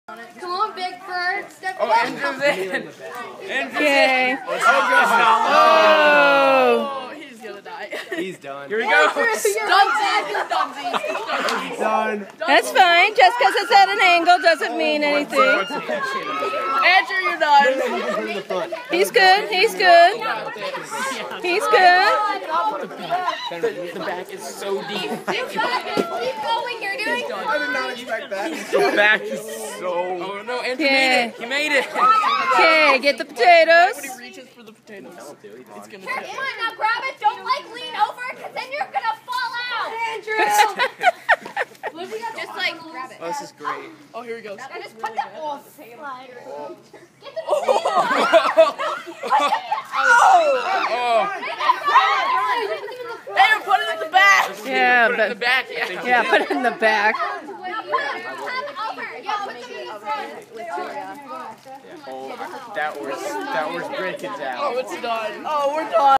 EDPS 8th Grade Big Bird Boat Float
After designing and creating a cardboard boat, the eighth graders had to complete their project by testing how well they worked. Students had to get their boat across the water, collect a 10 pound bag of cargo, and return back. While all boats successfully made the trip, not all captains remained dry!